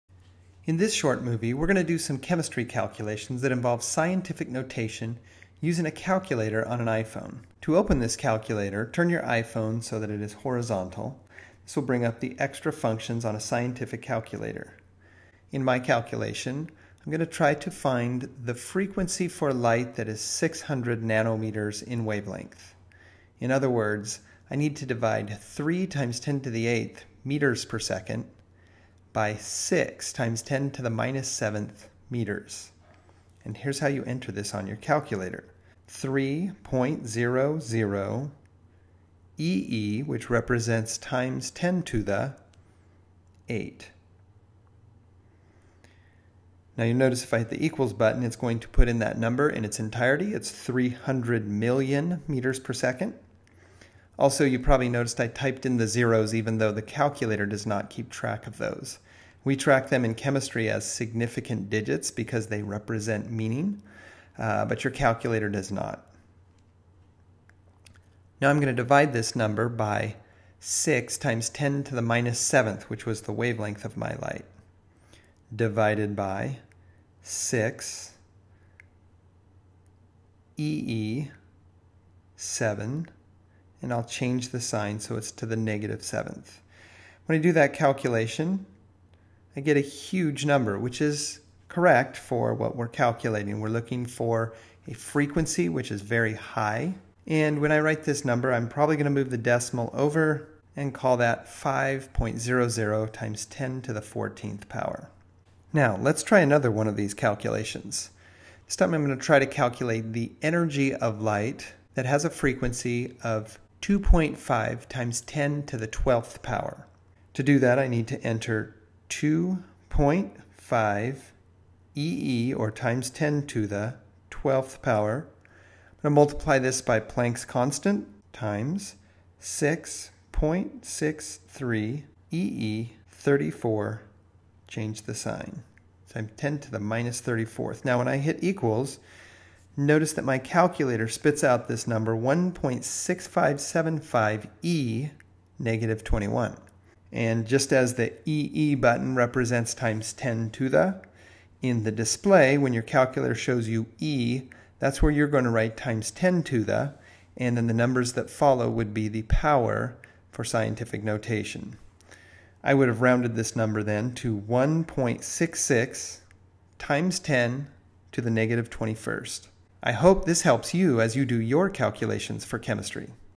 KL_Scientific Notation on iPhone
A short demonstration of typing numbers in scientific notation on an iPhone calculator